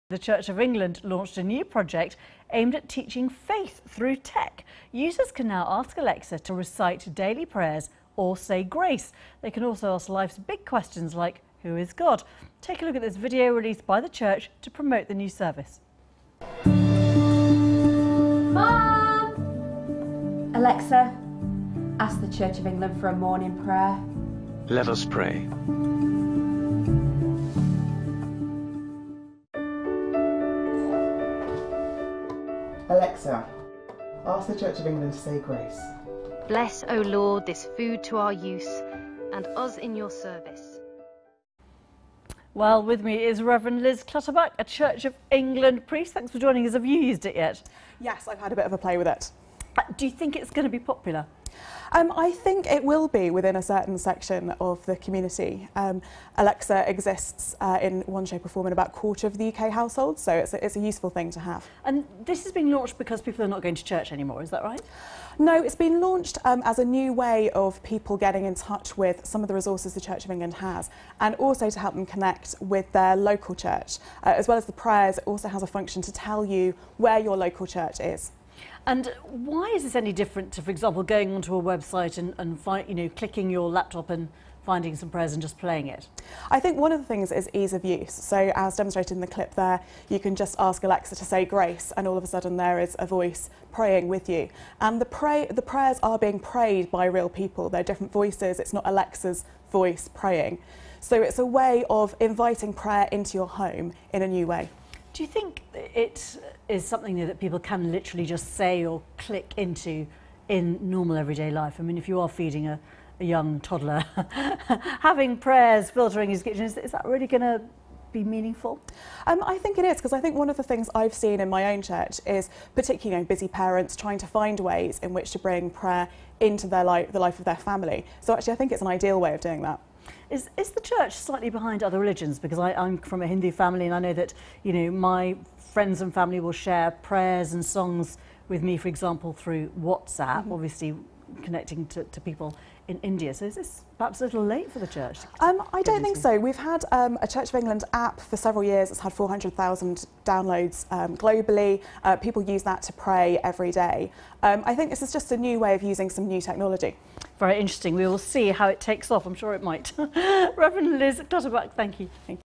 Church of England's Alexa skill on BBC World
Watch Revd Liz Clutterbuck talk to BBC World about the Church's Alexa skill launch.
Find out more about how the skill shares Christian resources and connects people with their local church